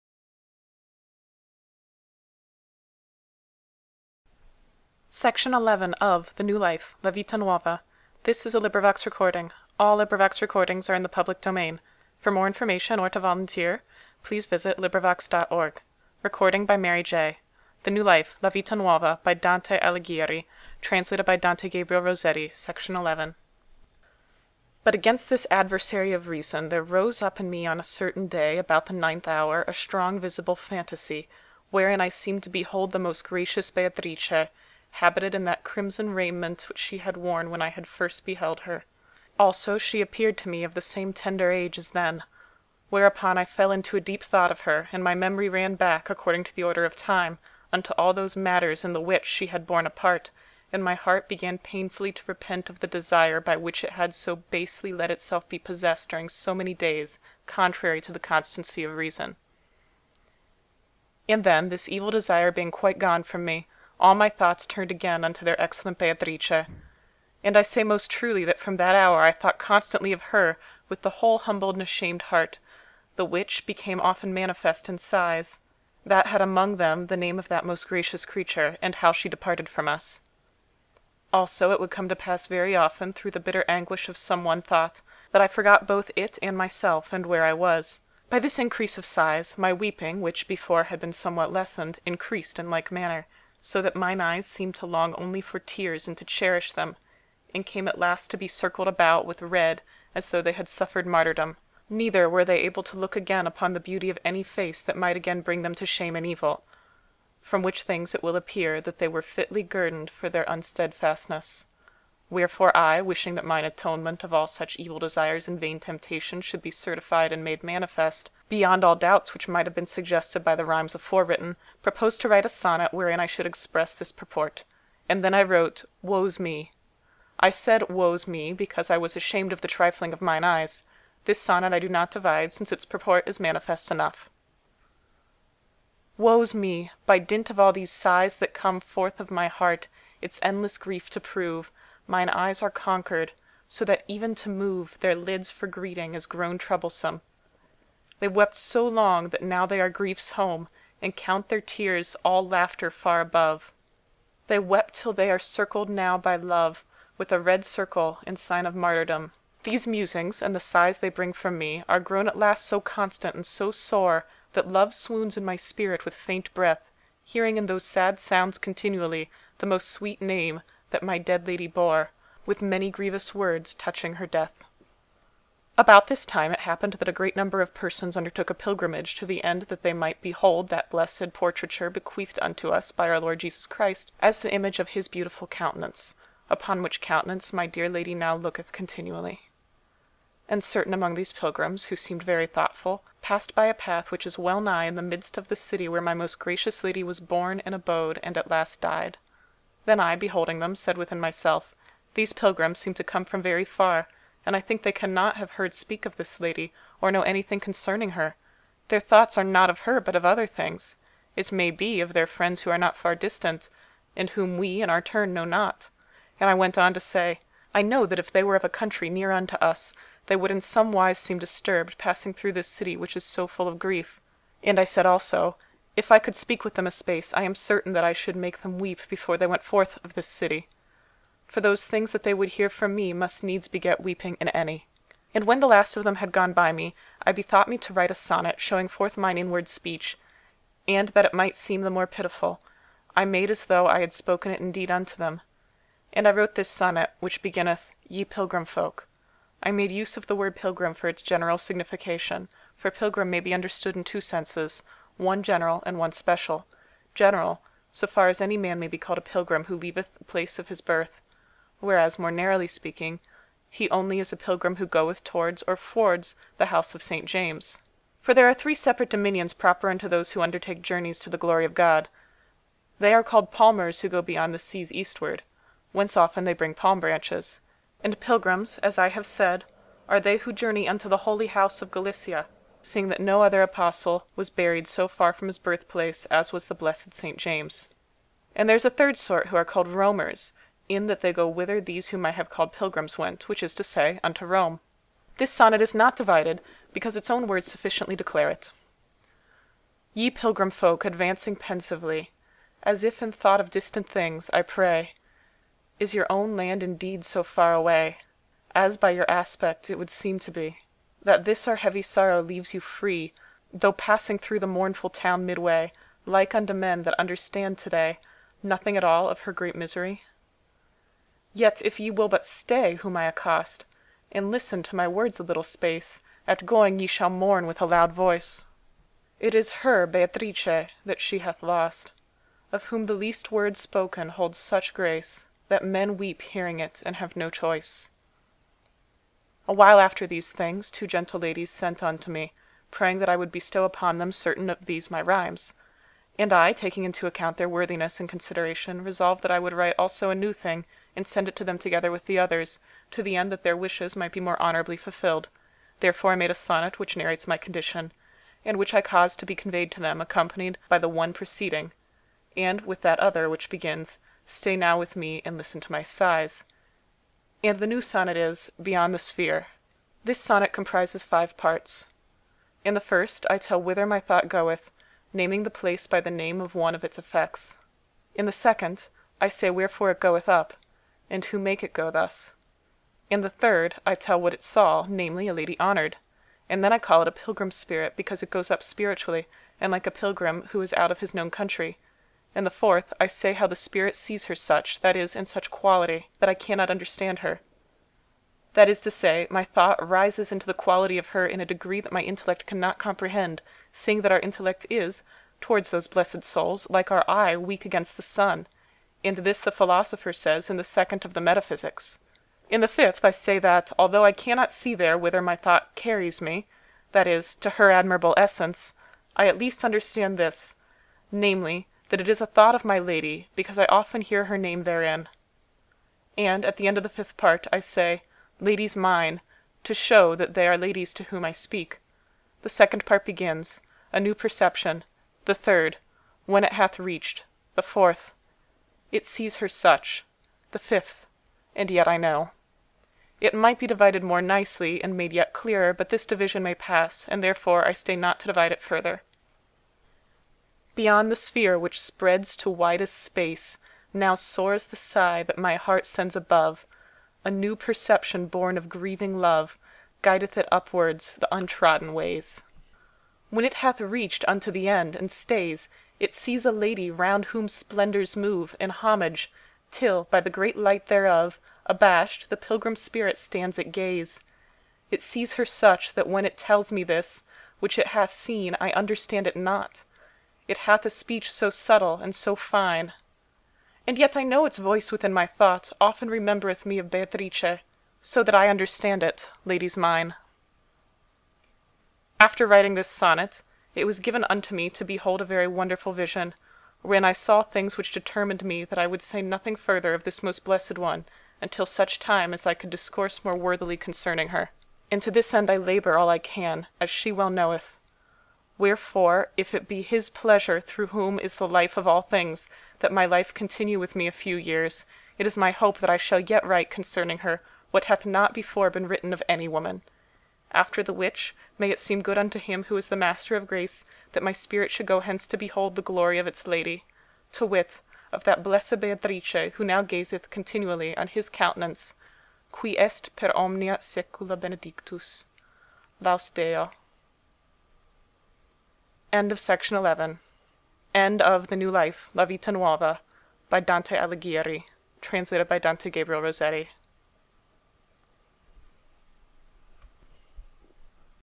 The New Life by Dante Alighieri - Section 11/11 (read by Mary J)
LibriVox recording of The New Life (La vita nuova) by Dante Alighieri. (Translated by Dante Gabriel Rossetti.) Read in English by Mary J.

Cover image: Dante Meets Beatrice at Ponte Santa Trinita by Henry Holiday, 1883. CD cover design by LA Walden.